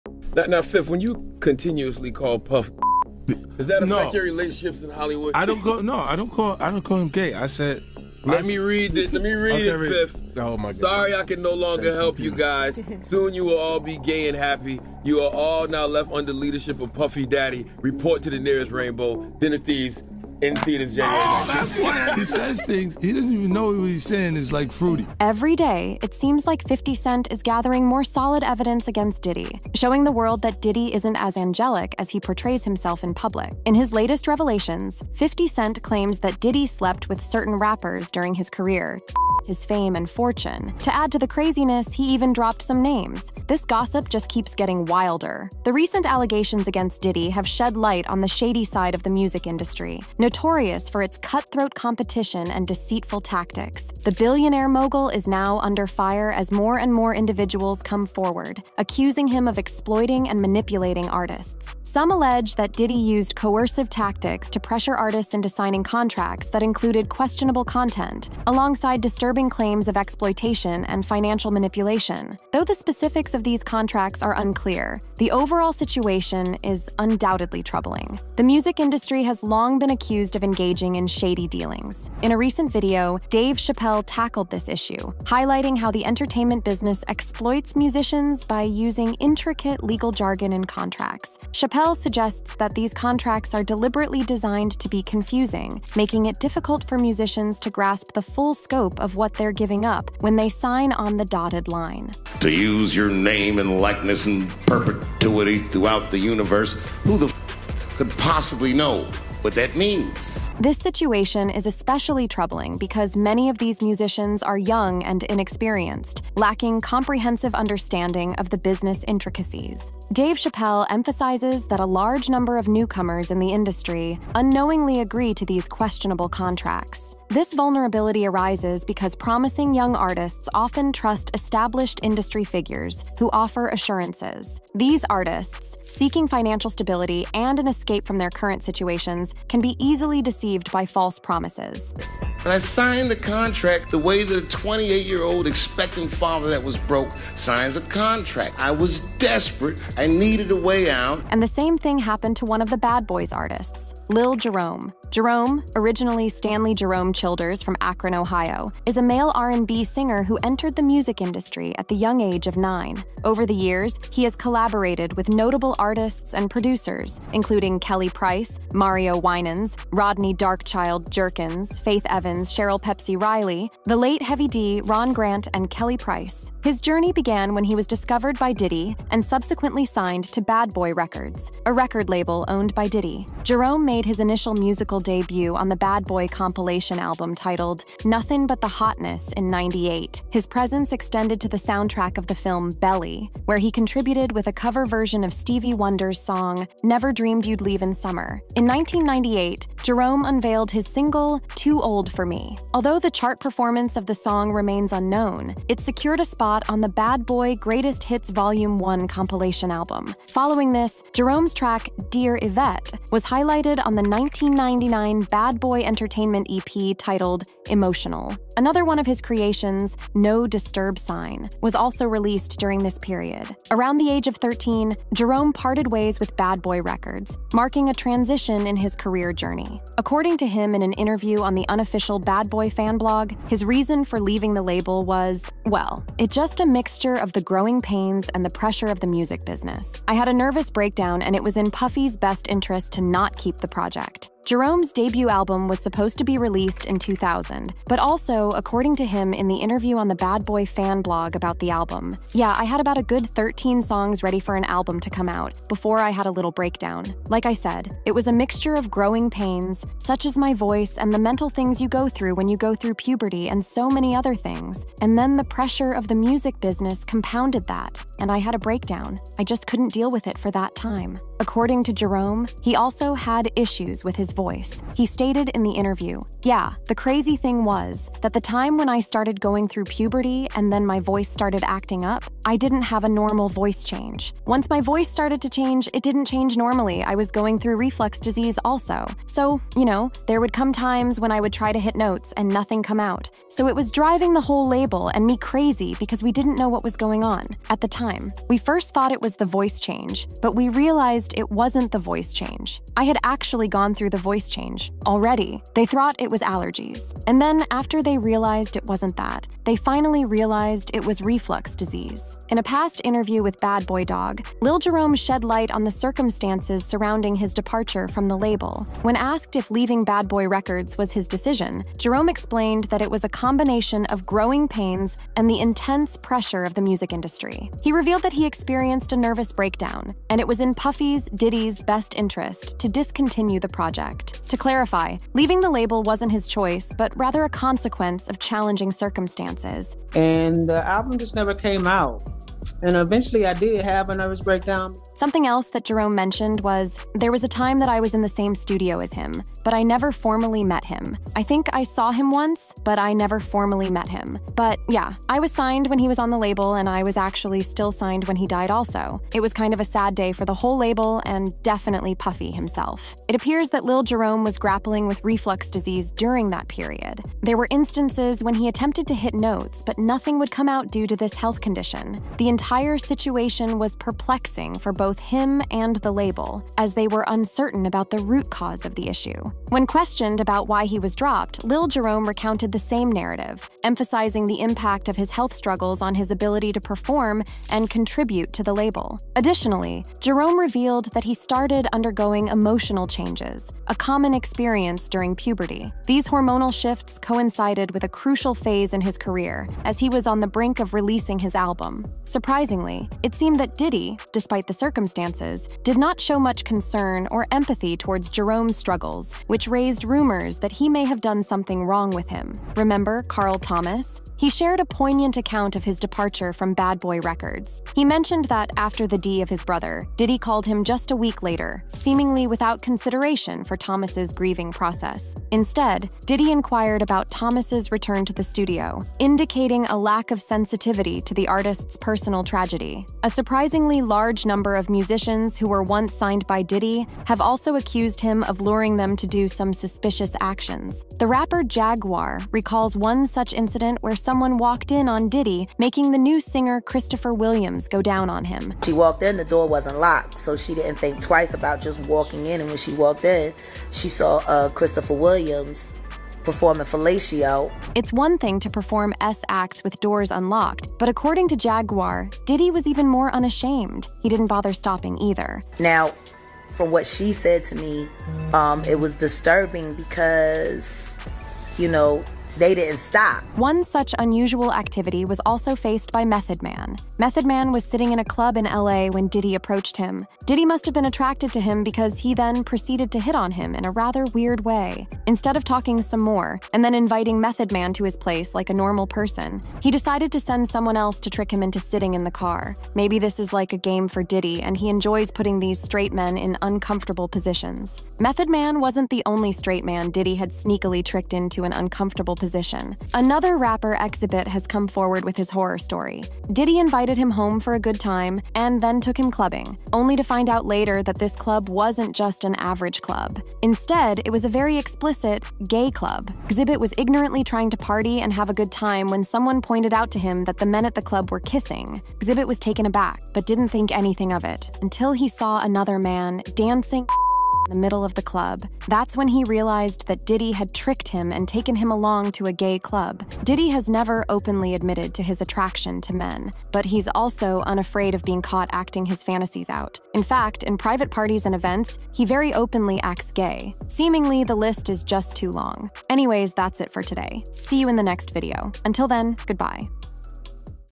50 Cent EXPOSES List Of Celebs Diddy SLEPT With..
50 Cent EXPOSES List Of Celebs Diddy SLEPT With..
Every day, it seems like 50 Cent is gathering more solid evidence against Diddy, showing the world that Diddy isn't as angelic as he portrays himself in public. In his latest revelations, 50 Cent claims that Diddy slept with certain rappers during his career to boost his fame and fortune. To add to the craziness, he even dropped some names. This gossip just keeps getting wilder!